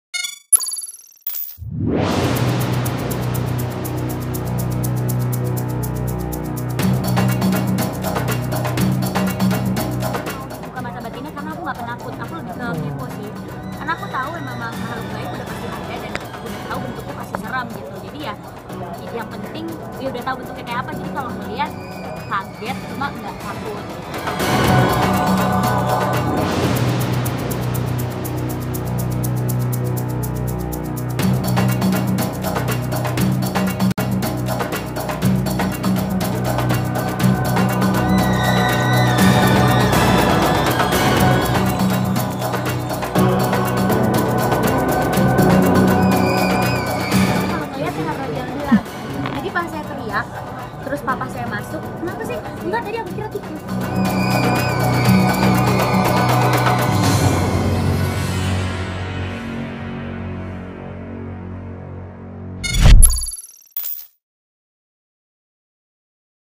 Mata Batinnya Dibuka, Prilly Sering Teriak-teriak Sendiri
Prilly Latuconsina membuka mata batinnya demi film horor yang ia bintangi. Kejadian apa saja ya yang Prilly alami?